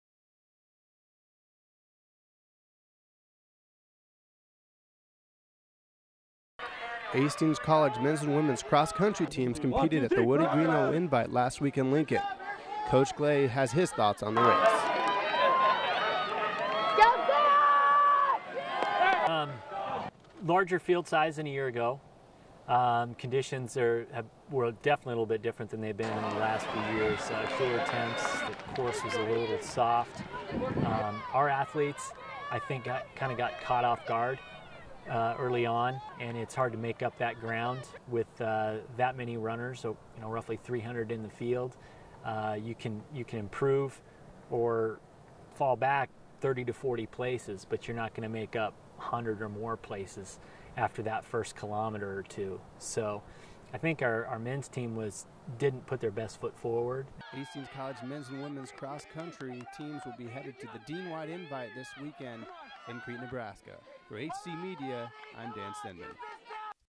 Hastings College Cross Country
Hastings College Women's and Men's Cross Country Teams are in action at the Woody Greeno Invite in Lincoln, Nebraska